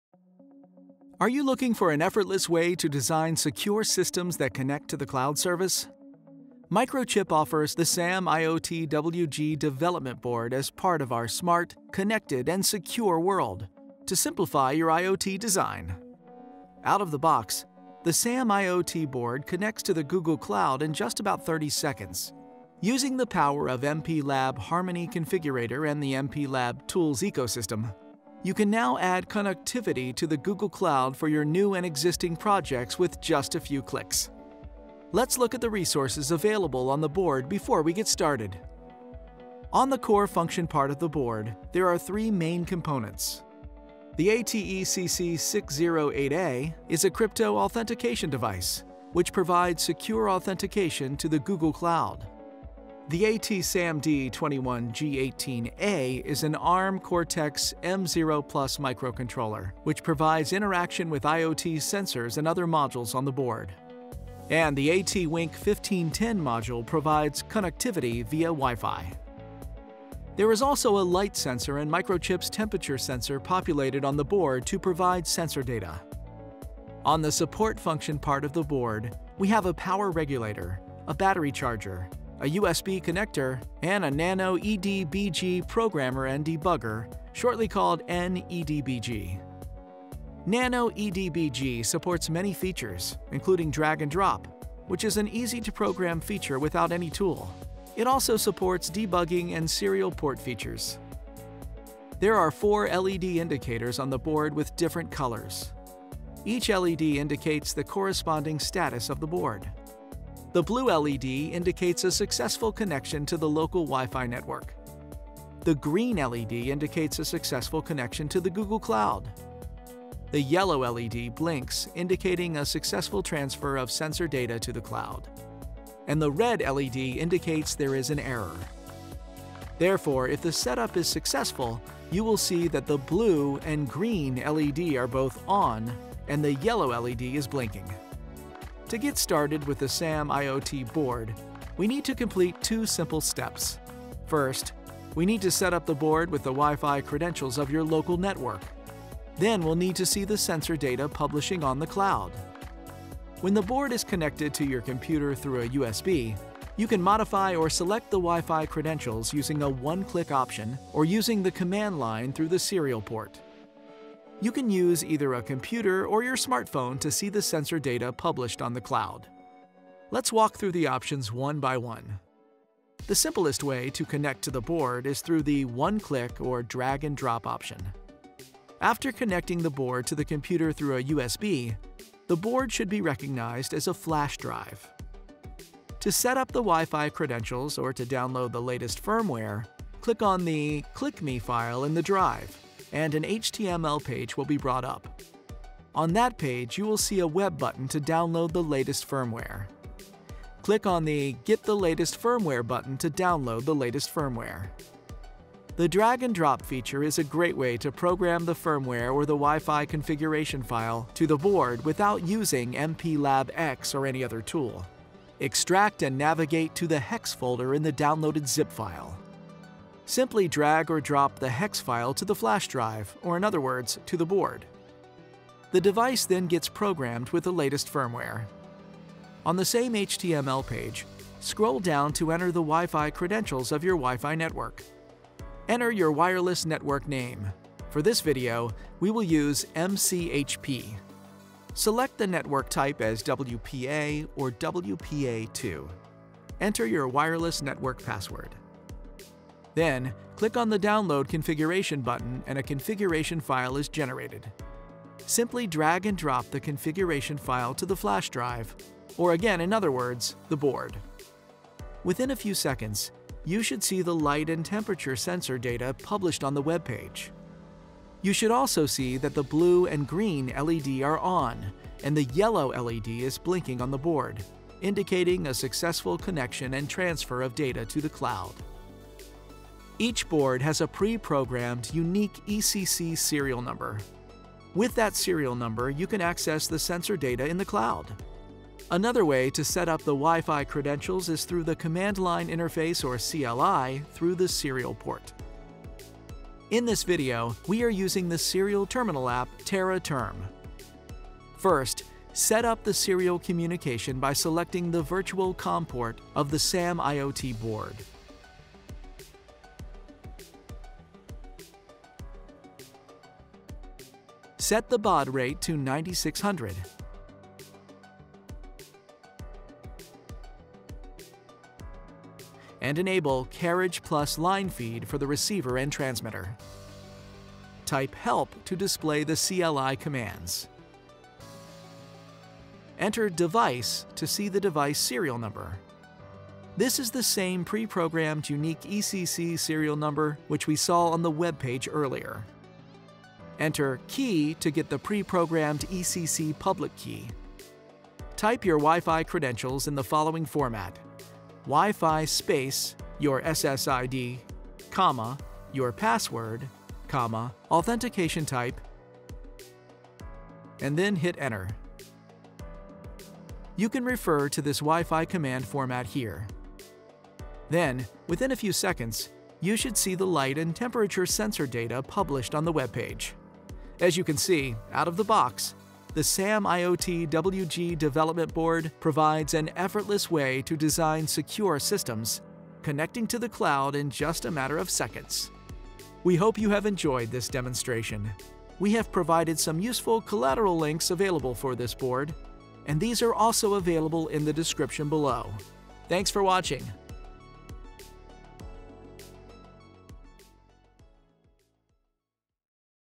Getting Started with the SAM IoT WG Development Board
This video covers the easy steps to get started with the SAM-IoT WG Development Board and the features available on the board.

The SAM-IoT WG Development Board features the SAMD21G18 Arm® Cortex®-M0+ based 32-bit microcontroller (MCU), an ATECC608A CryptoAuthentication™ secure element IC and the fully certified ATWINC1510 Wi-Fi® network controller, so you can quickly and easily connect your embedded application to Google’s Cloud IoT core platform. The on-board debugger allows you to program and debug the MCU without any additional hardware. Use the mikroBUS™ sockets to expand your design with your choice of MikroElekronika click Boards™. The SAM-IoT WG development board is a small and easily expandable demonstration and development platform for IoT solutions.

Out of the box, the MCU comes preloaded with firmware that enables you to quickly connect and send data to the Google Cloud Platform using the on-board temperature and light sensors. Once you are ready to build your own custom design, you can easily generate code using the free software libraries in MPLAB Harmony v3. The SAM-IoT WG Development Board is supported by MPLAB® X IDE.